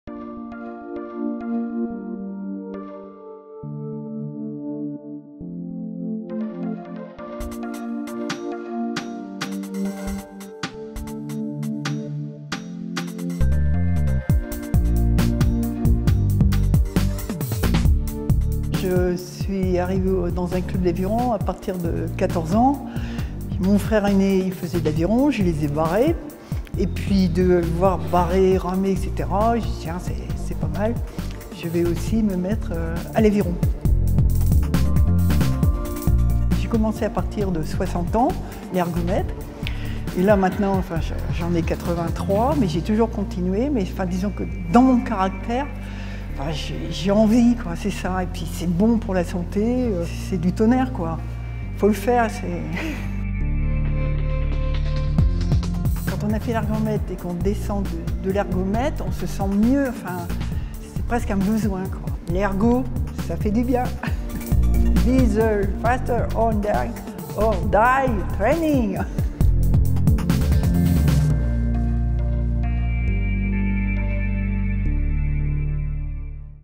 En route pour les Mondiaux d'Aviron Indoor : Portrait #4 Renée Camu
Les prochains championnats du monde d'Aviron Indoor auront lieu à Paris en février 2020. De l'élite, aux crossfiters en passant par les passionnés de l'ergomètre, découvrez tous les mois un portrait d'un ou d'une athlète pratiquant la discipline.